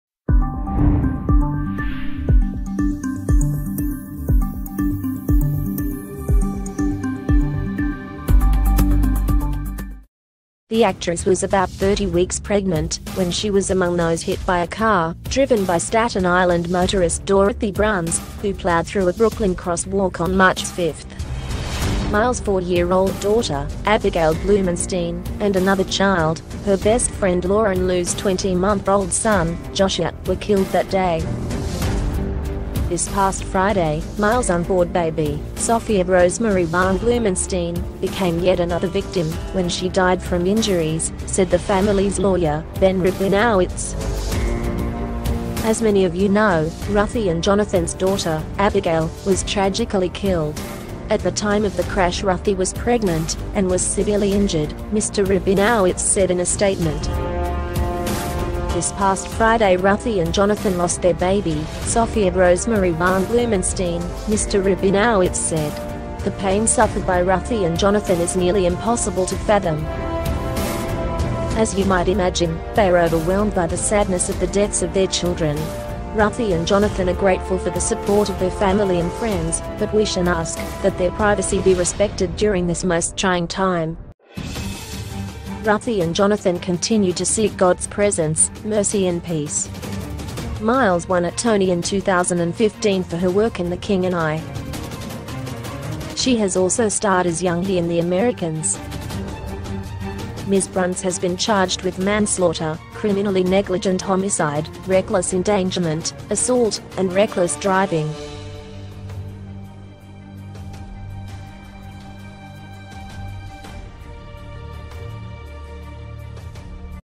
Tony winning star Ruthie Ann Miles loses months after daughter killed in Brooklyn
The actress was about 30 weeks pregnant when she was among those hit by a car driven by Staten Island motorist Dorothy Bruns, who ploughed through a Brooklyn crosswalk on March 5.
[time]Miles’ four-year-old daughter, Abigail Blumenstein, and another child — her best friend Lauren Lew’s 20-month-old son, Joshua — were killed that day.